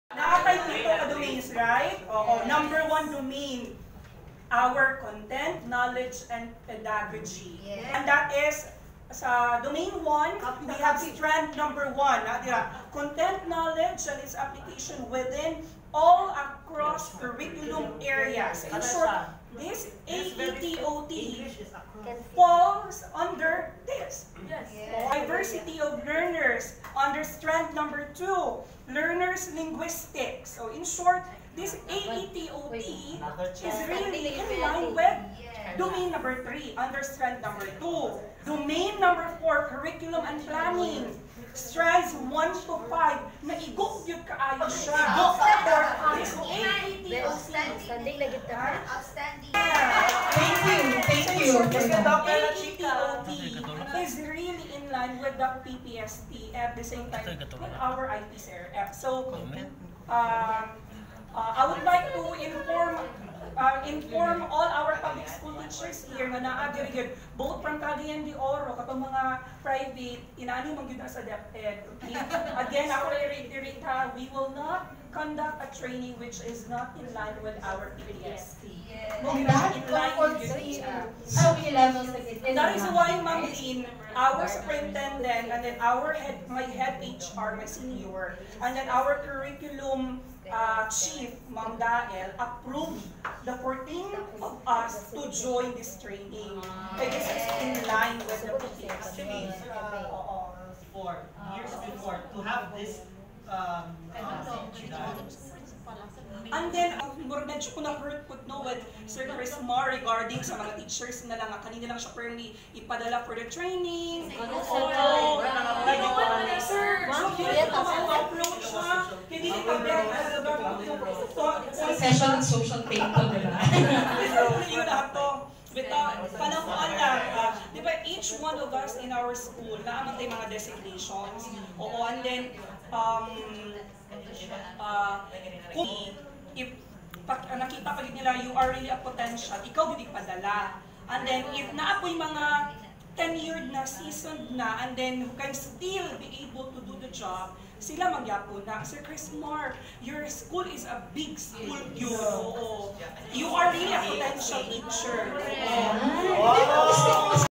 TEACHER'S TRAINING FOR IPCRF | American English Training of Trainers 2019
Sharing you this vid where Sir Eman, EPS for Human Resource explained the relevance of American English Training of Trainers for DepEd participants in light of the IPCRF.

Wow!